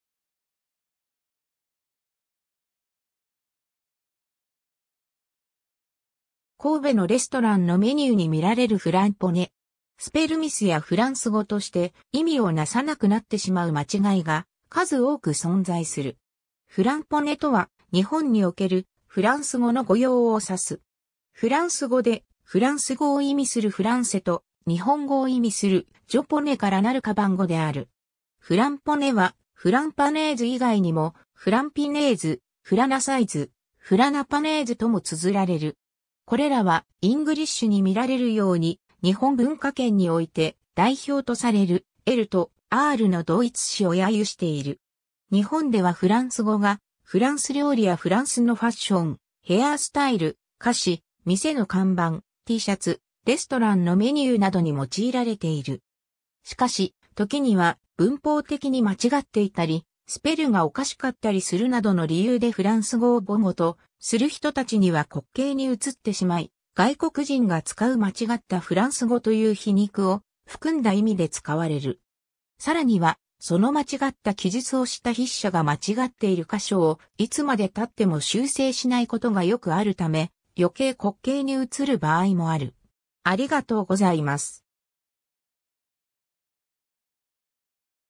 フランポネ
神戸のレストランのメニューに見られるフランポネ。
スペルミスやフランス語として意味を成さなくなってしまう間違いが数多く存在する。
 フランポネ (フランス語: franponais) とは、日本におけるフランス語の誤用を指す。
フランス語で、フランス語を意味するfrançaisと日本語を意味するjaponaisからなるかばん語である。
フランポネは「franponais」以外にも「framponais」、「flançais」、「flanponais」とも綴られる。
これらはEngrishに見られるように、日本文化圏において代表とされる、「L」と「R」の同一視を揶揄している。
 日本ではフランス語がフランス料理やフランスのファッション、ヘアースタイル、菓子、店の看板、Tシャツ、レストランのメニューなどに用いられている。
 しかし、時には文法的に間違っていたり、スペルがおかしかったりするなどの理由でフランス語を母語とする人たちには滑稽に映ってしまい、「外国人（この場合日本人）が使う間違ったフランス語」という皮肉を含んだ意味で使われる。
更にはその間違った記述をした筆者が間違っている箇所をいつまでたっても修正しないことがよくあるため、余計滑稽に映る場合もある。